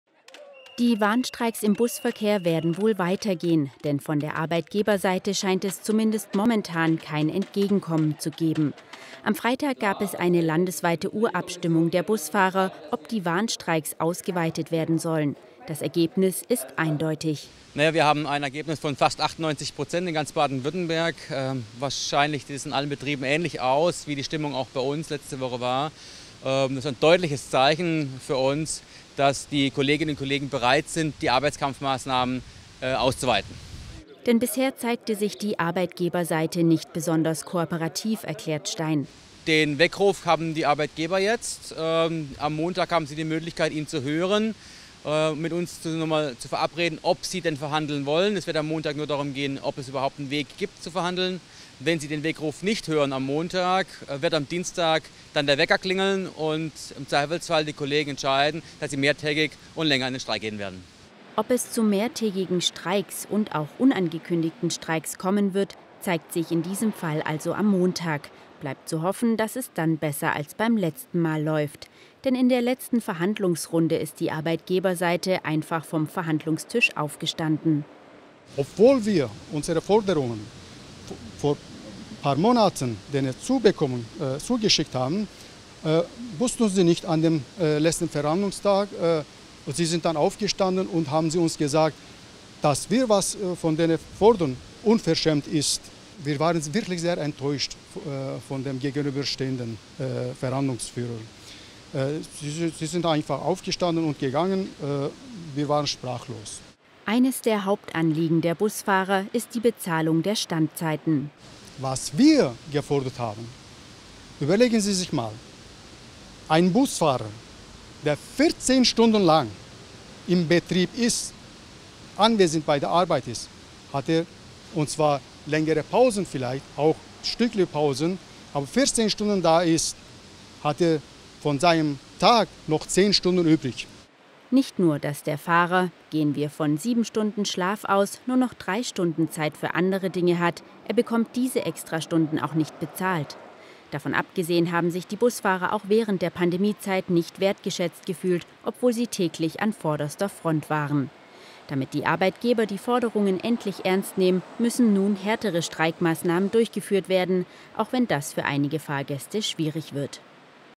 Warnstreiks im Busverkehr - 98% der Busfahrer wollen weiter streiken
RTF.1 Nachrichten: Warnstreiks im Busverkehr - 98% der Busfahrer wollen weiter streiken